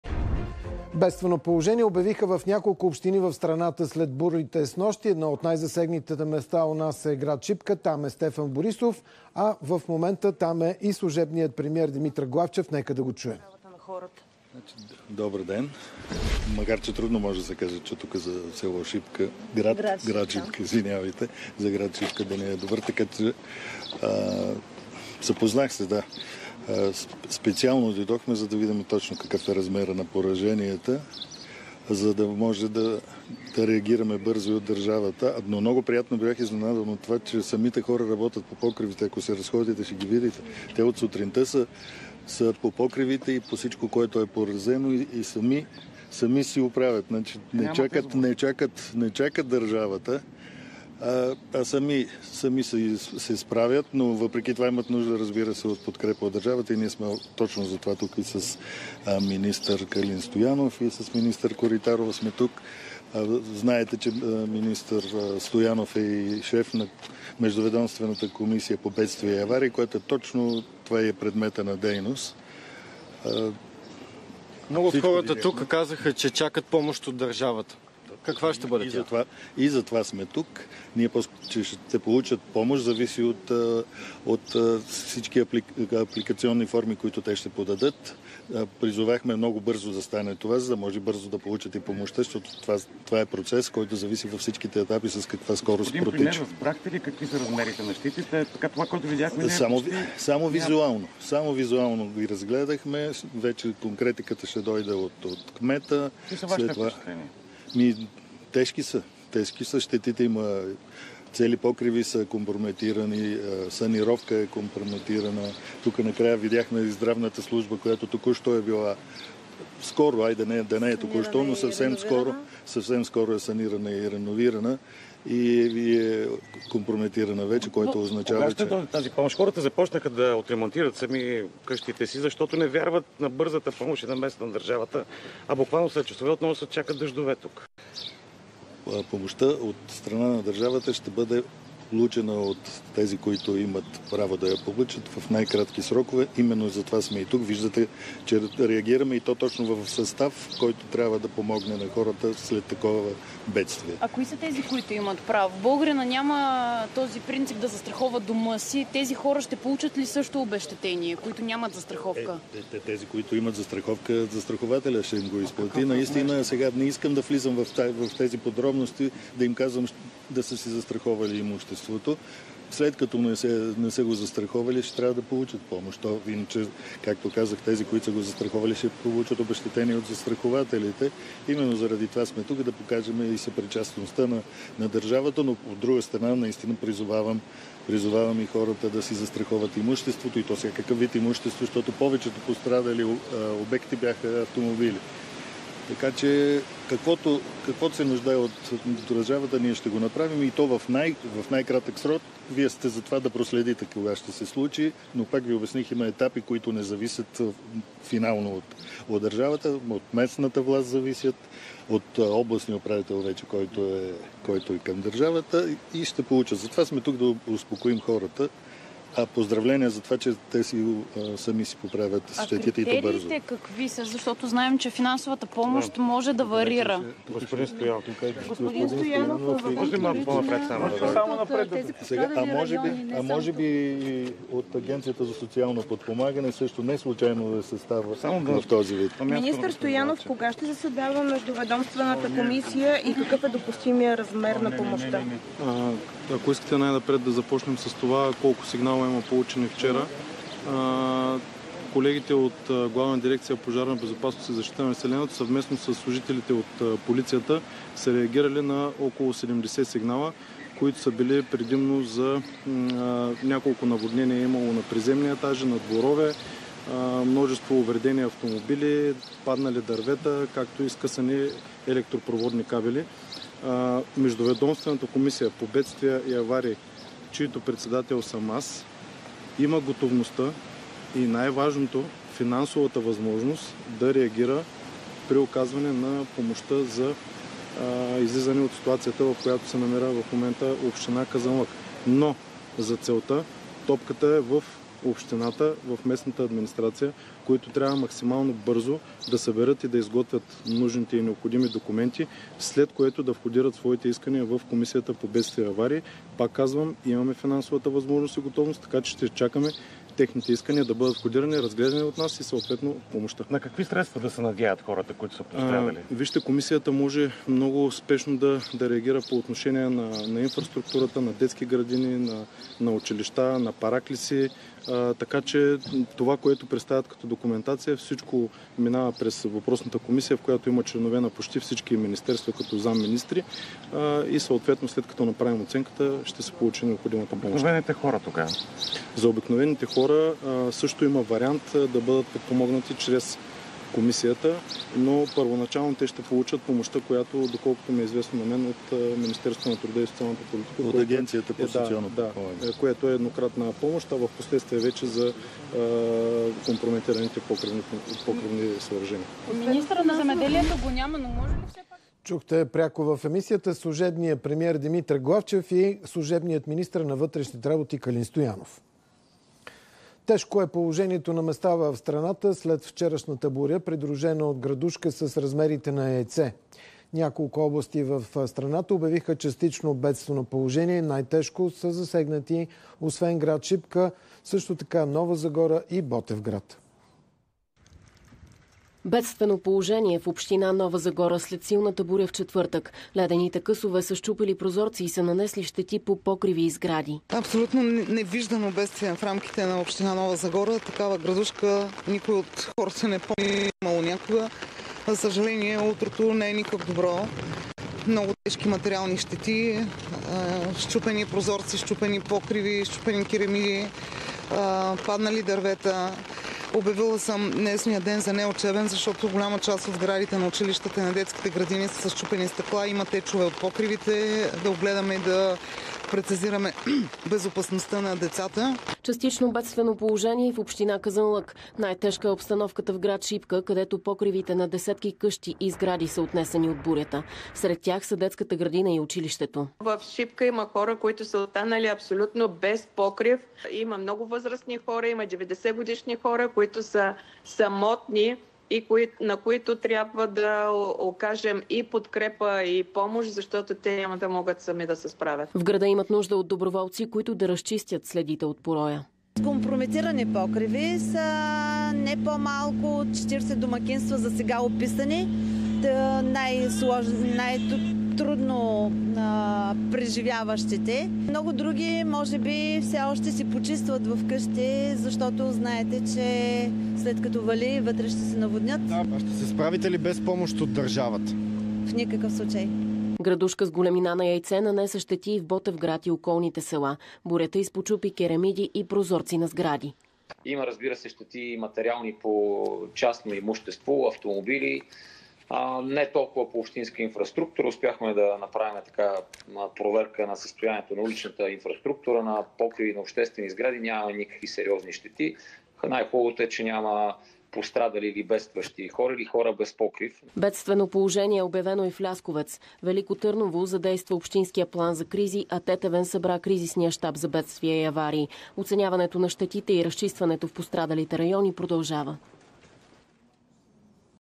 След опустошителната градушка: Главчев обеща бърза реакция от държавата
„Тежки са щетите. Цели покриви са компрометирани, санировка е компрометирана, тука видяхме и здравната служба, която скоро е санирана и реновирана, и днес отново се нуждае от ремонт. Помощта от държавата ще бъде получена от тези, които имат право в най-кратки срокове. Каквото зависи от държавата, ще го направим в най-бързи етапи“, каза Главчев от гр. Шишка, който пострада тежко от опустошителна градушка.